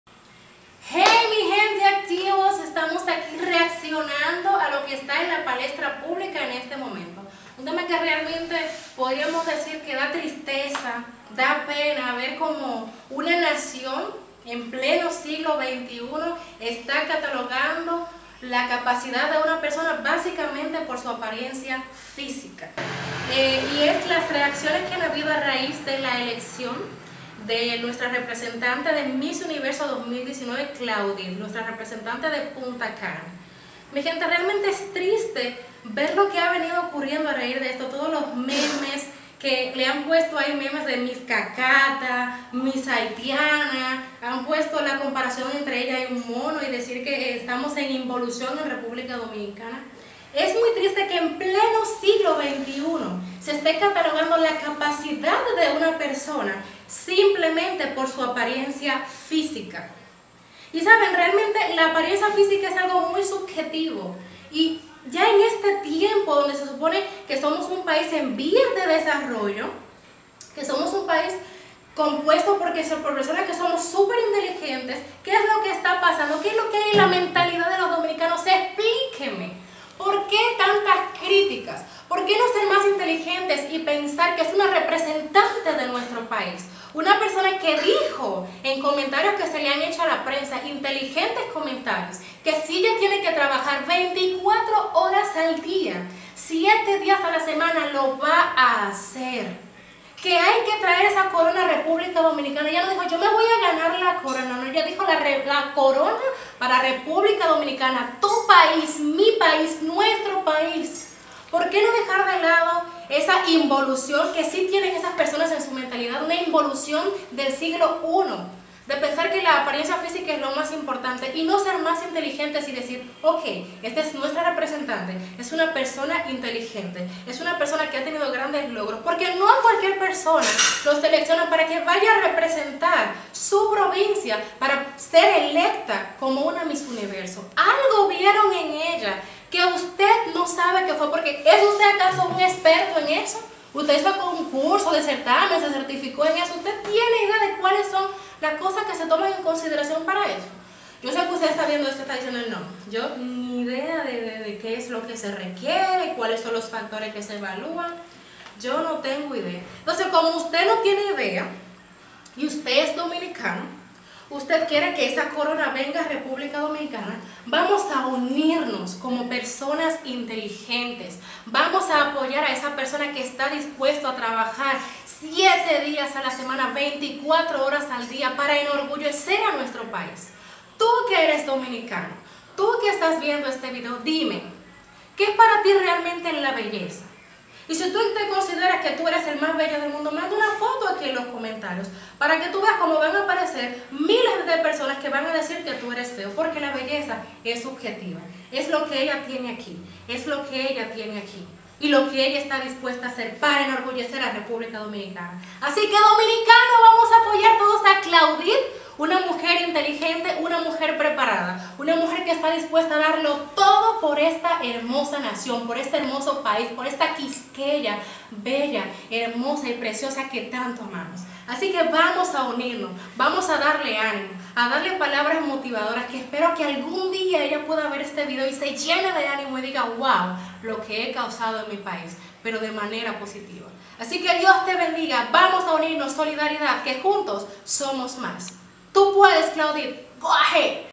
Miss Republica Dominicana 2019, Clauvid Daly ¿Es fea?
Miss Republica Dominicana 2019, Clauvid Daly, un caso que ha causado mucho revuelo, pues hemos visto un pais que tristemente en vez de unirse y apoyar a nuestra representante, se ha puesto a una misma voz para criticarla y atacarla.